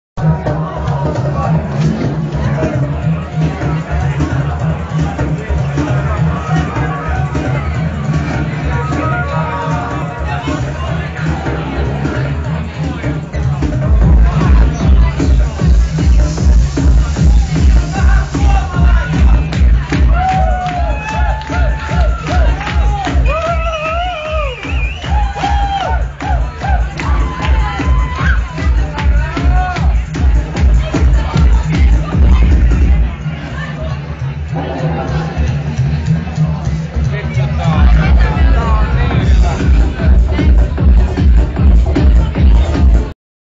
Beat vs Rally, Ilokivi/Jkl: Yläkerran deejii
Ilokiven yläkerran tanssit

Beat vs Rally, Ilokivi - Jyväskylä 4.8.2007

Järjesti Efekti ry